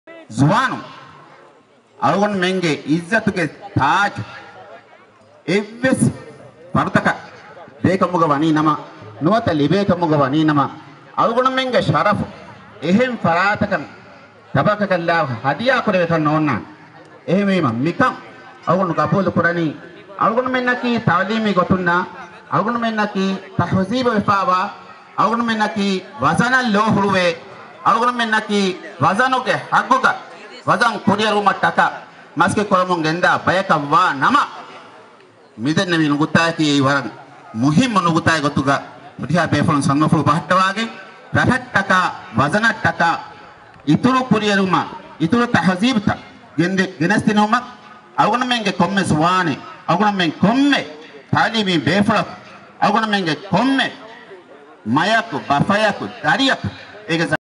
2013 Vana aharuge Maicup Feshumuge rasmiyaathuge therein (Mohamed Ibrahim ge Vaahakakolhu)
2013 vana aharuge Maicup football mubaaraayi feshumuge rasmiyyaathugai Mohamed Ibrahim Dhehkevi vaahakakolhu.